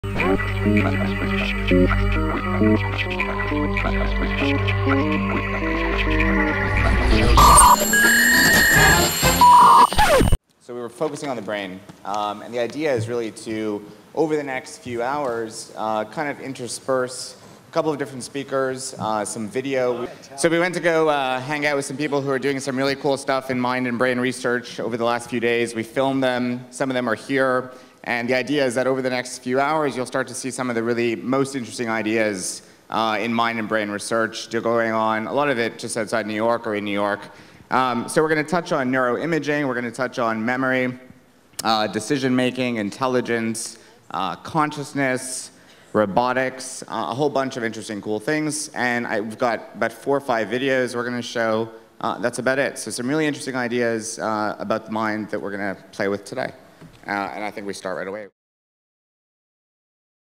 Gadgetoff 2007 - Adam Bly
Adam introduces a number of interesting videos that detail the latest advancements in the understanding of the brain.